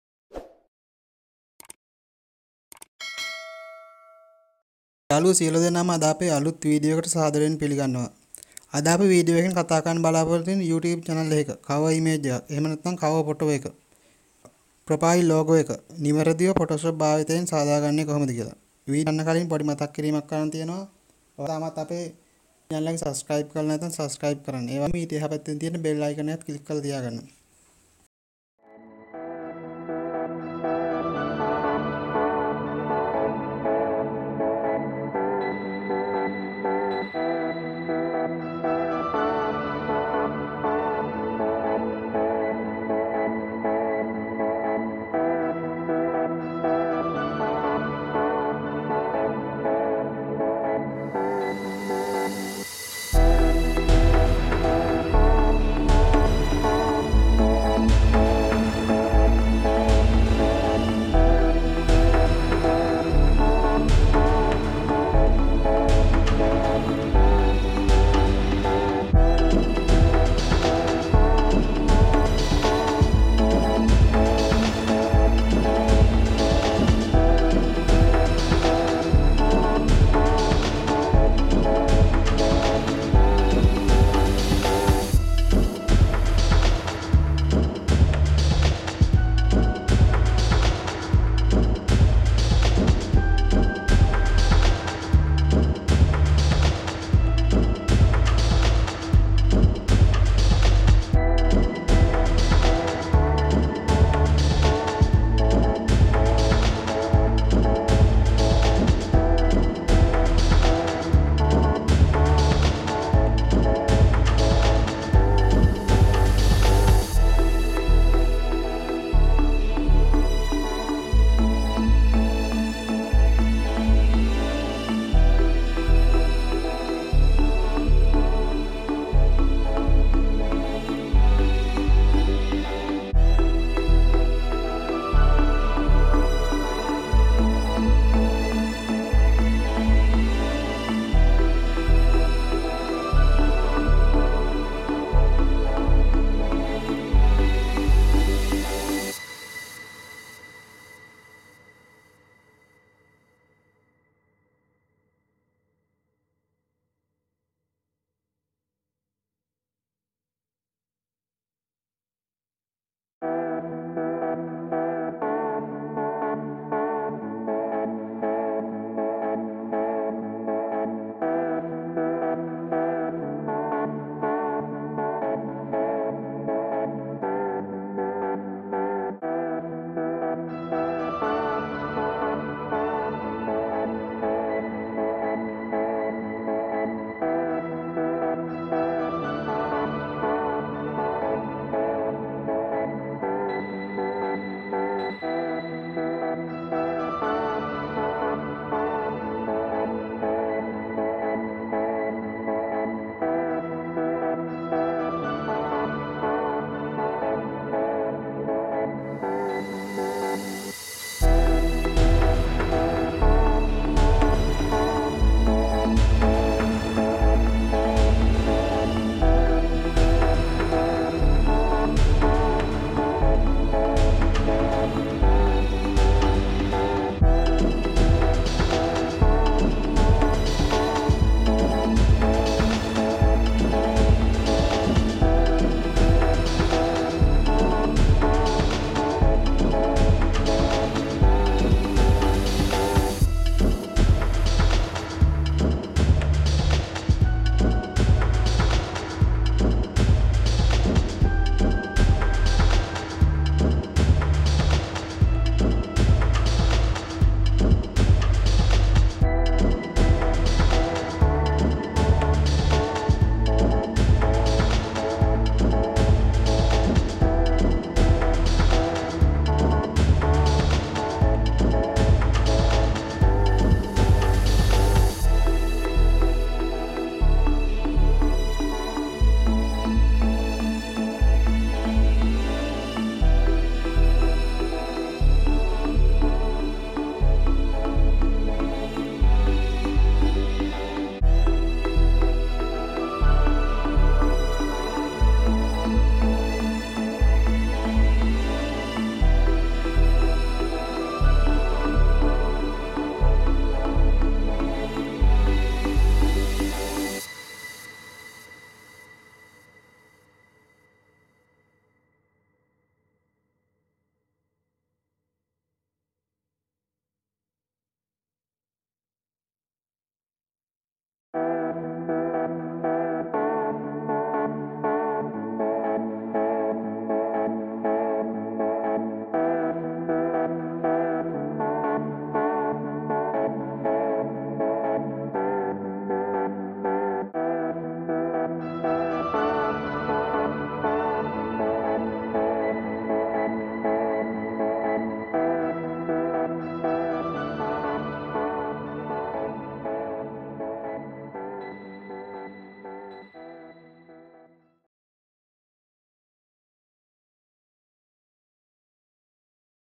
| Photoshop Tutorial | YouTube channel art and logo | Dushan Online |
Hi, friends Today in this video I'm showing how to make a YouTube channel art and logo for your YouTube channel using Photoshop.